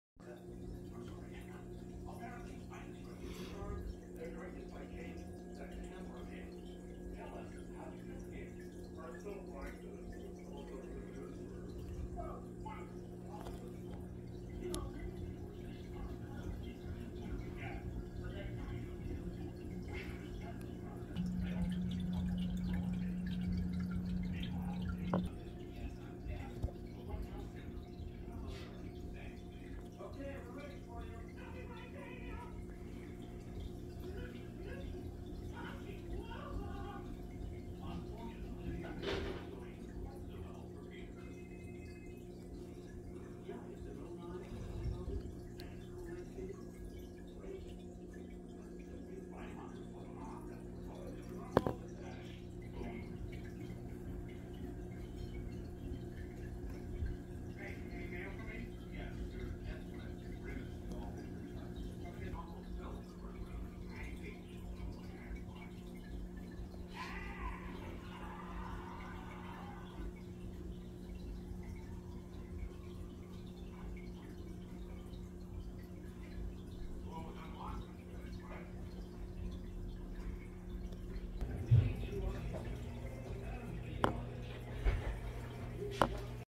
Filming my fish
today I film my pet fish Gibson